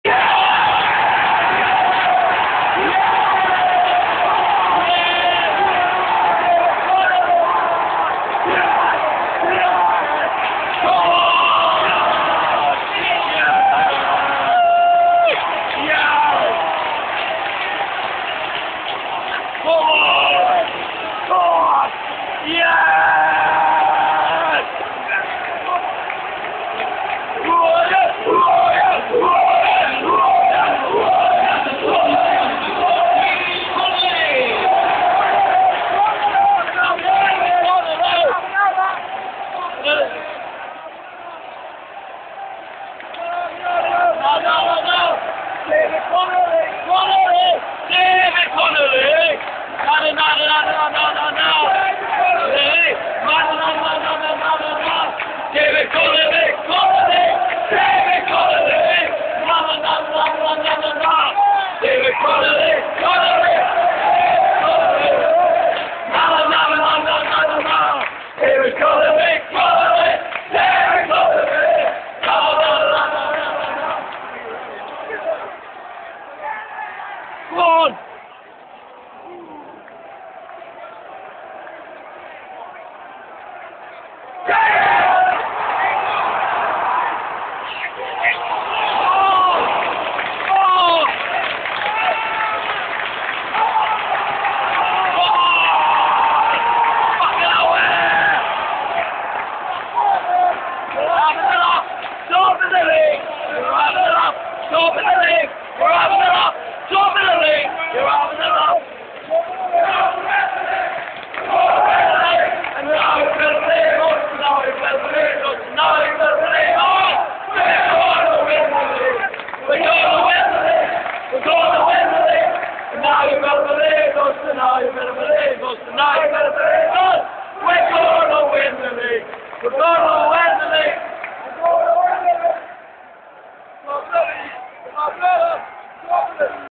safc 06/07
sunderland afc vs derby county...hahaha...connollys penalty, sorry bwt th camera action, i woz 2 exicted